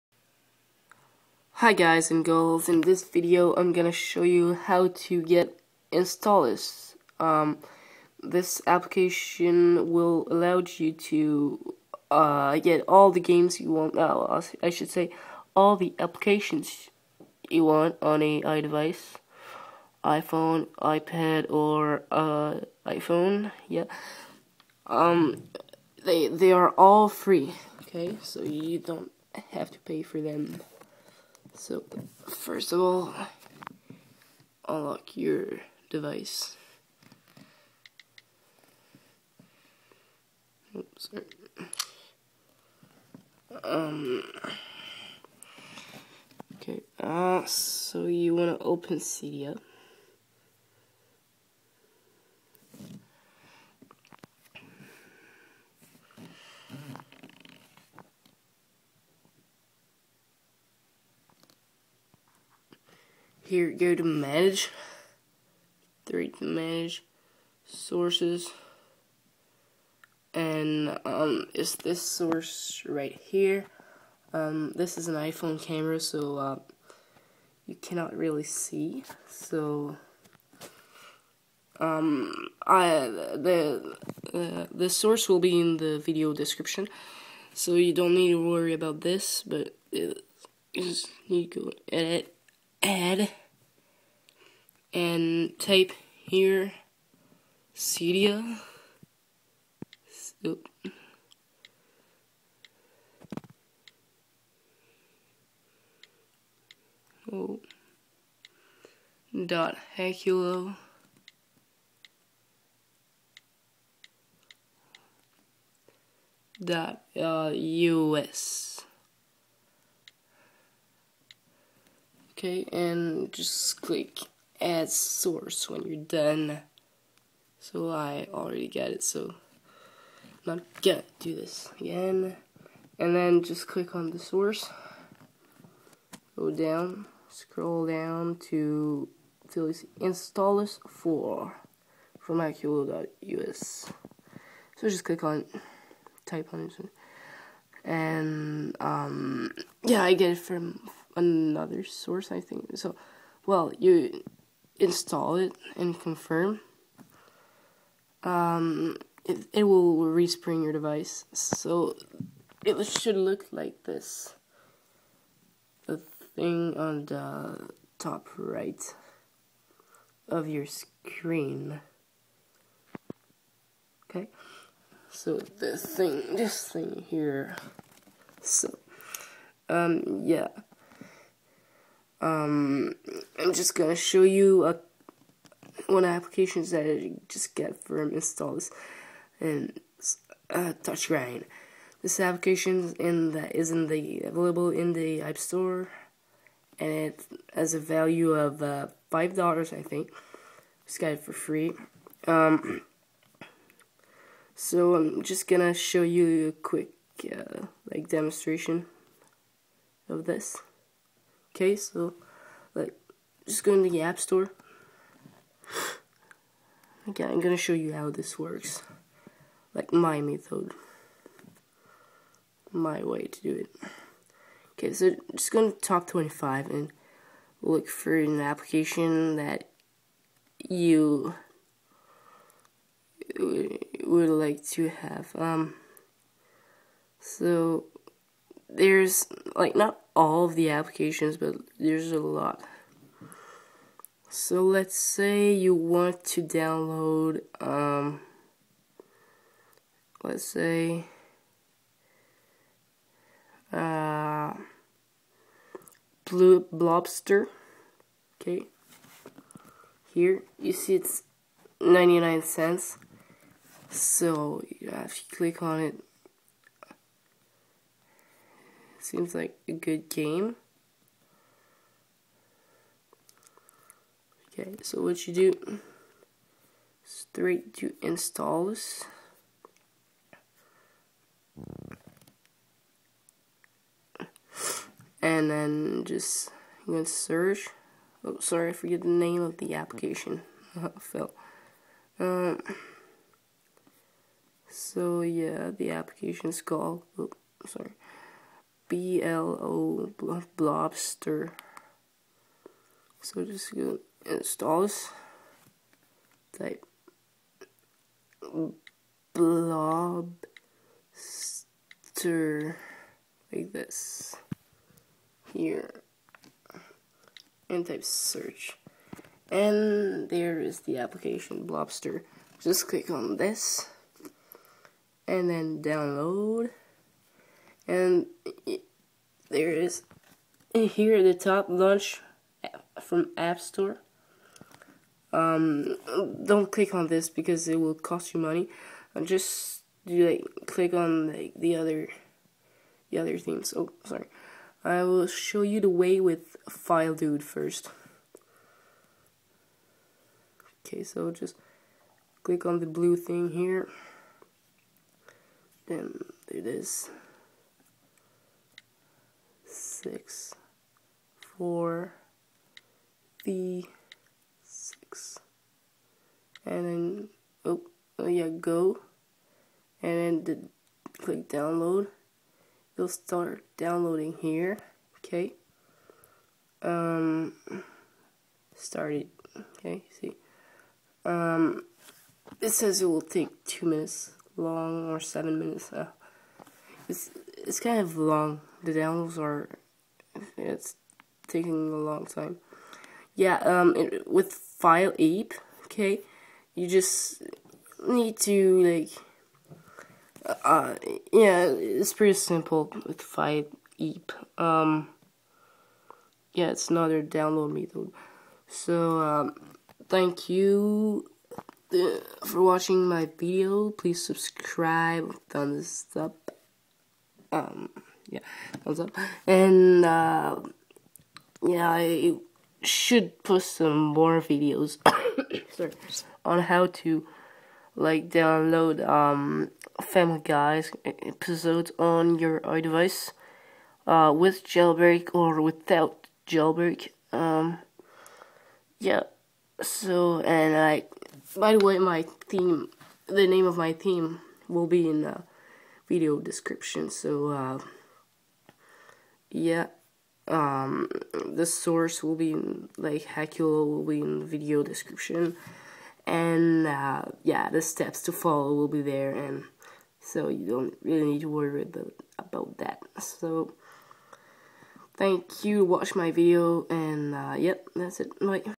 How to get installous from cydia (free apps)
This is only for jailbroken device.

1.Open your iDevice.

2.Open Cydia ( the app that comes with the jailbreak.

3.Go to manage then sources.

4. Press edit ( top right ) then add ( top left )

5. The source is     Cydia.hackulo.us

6.then search for     Installous 4

This is how to use Installous 4:

1. Go to search.

2. Type the name of the app that you want to download.

3. Press download.

4. Follow the instructions in the video to download.

5. After the download install the app.

The name of my theme is: A UINique HD Theme. From the source : repo.insanelyi.com